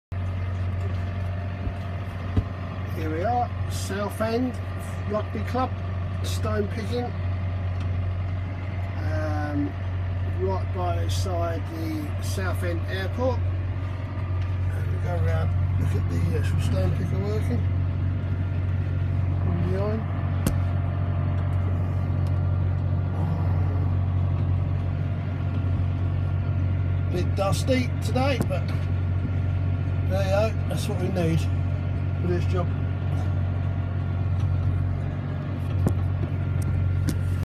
Stone collecting at Southend Rugby Club
JD 6125 tractor using a kivi pekka stone collector on a rugby pitch in Southend